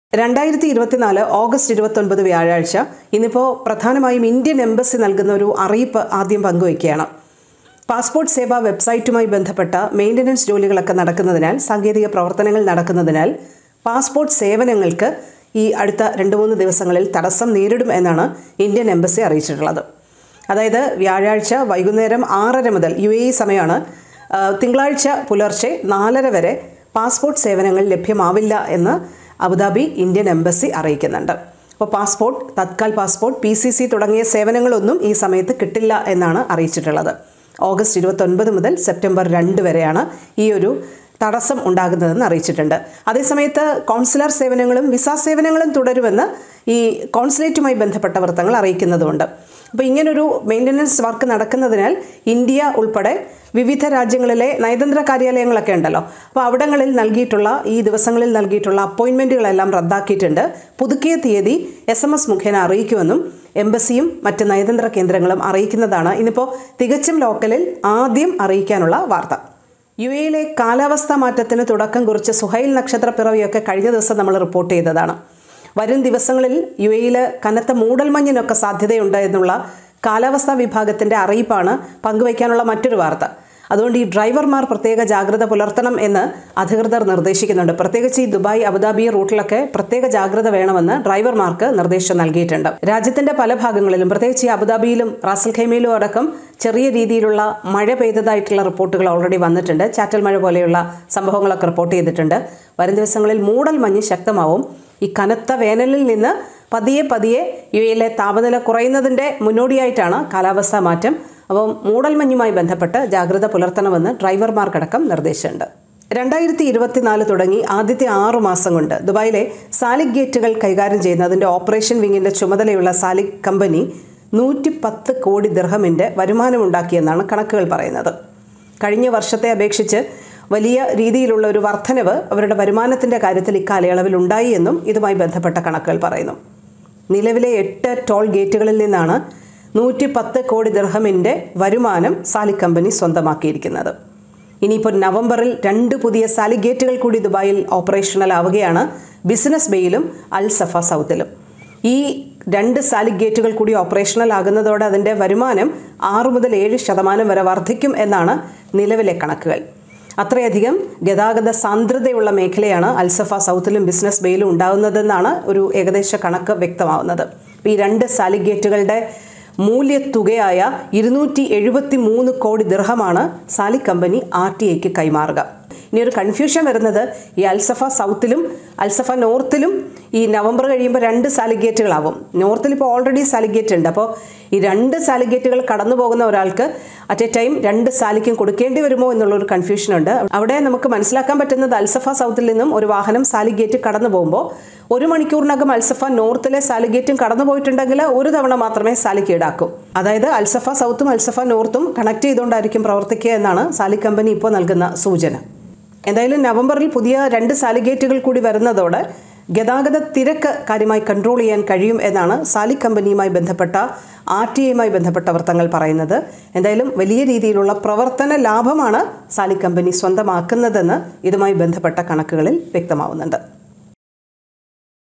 ഇന്ത്യൻ എംബസിയുടെ പാസ്പോർട്ട് സേവനങ്ങൾ തടസ്സപ്പെടും | യുഎഇയിലെ ഈ കാലാവസ്ഥ മാറ്റം ശ്രദ്ധിക്കണം
ഇന്ത്യൻ എംബസിയുടെ പാസ്പോർട്ട് സേവനങ്ങൾ തടസ്സപ്പെടും | യുഎഇയിലെ ഈ കാലാവസ്ഥ മാറ്റം ശ്രദ്ധിക്കണം | സാലിക് ഗേറ്റുകൾ പുതുതായി പ്രവർത്തന സജ്ജമാകുമ്പോൾ | തികച്ചും ലോക്കൽ